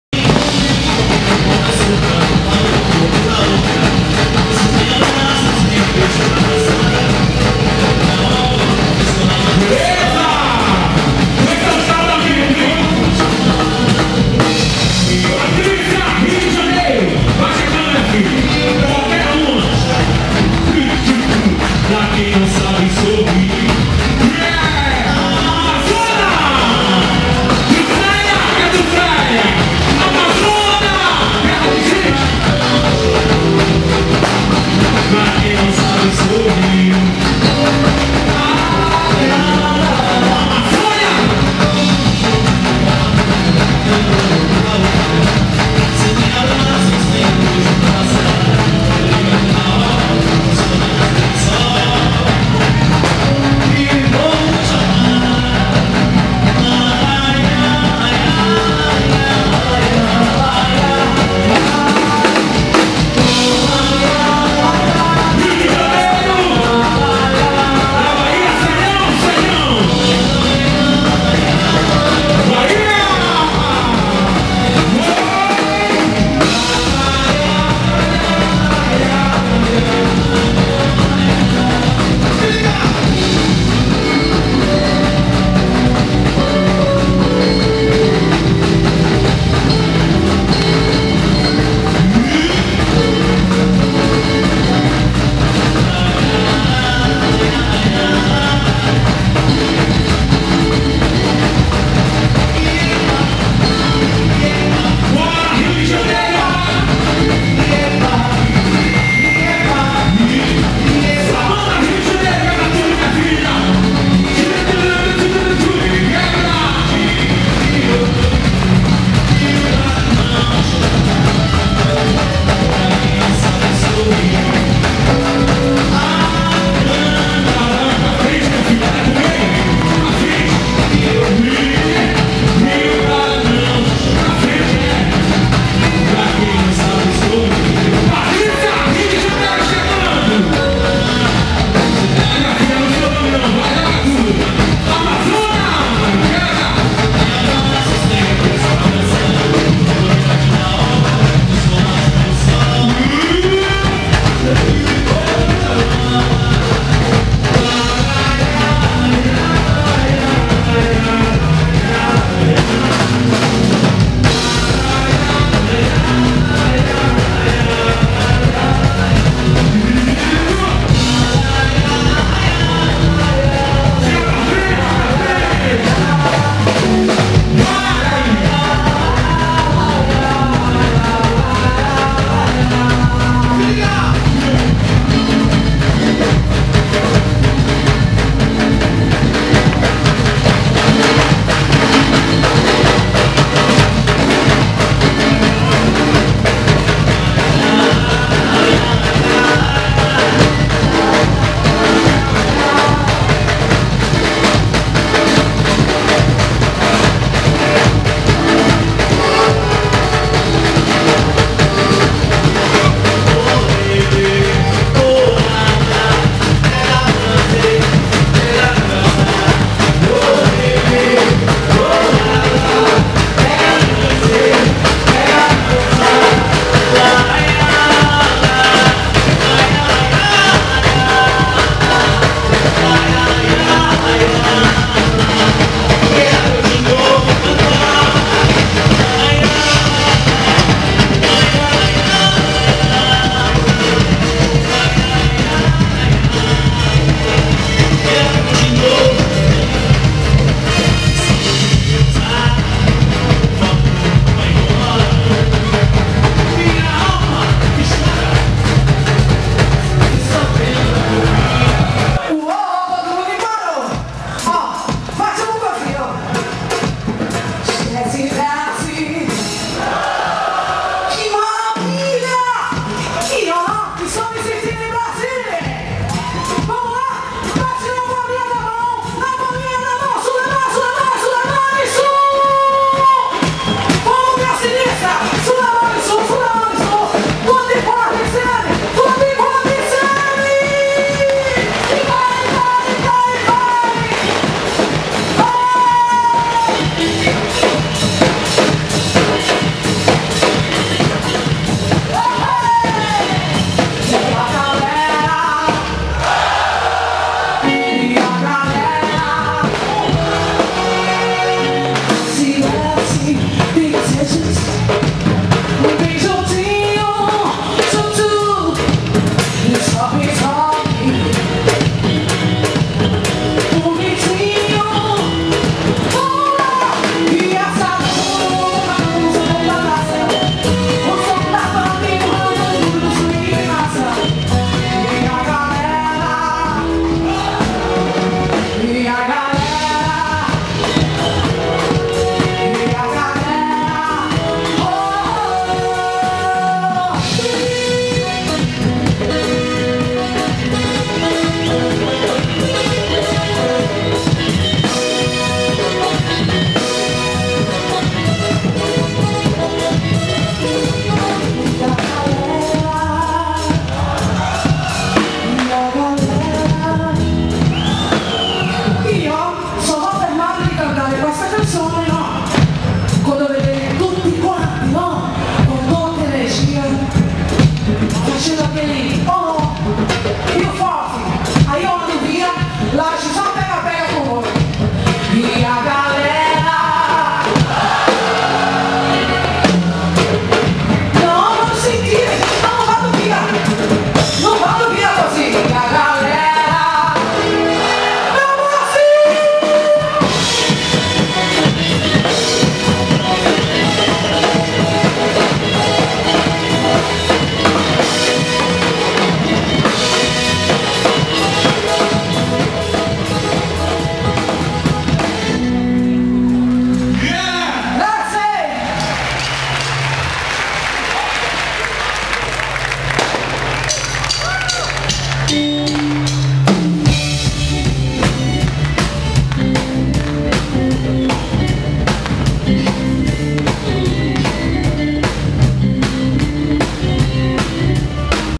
Fine Anno 2006 4°
Ritmi e musica dal Brasile, fanno da protagonisti al primo Capodanno in Piazza a San Benedetto Del Tronto, organizzato dall'amministrazione comunale.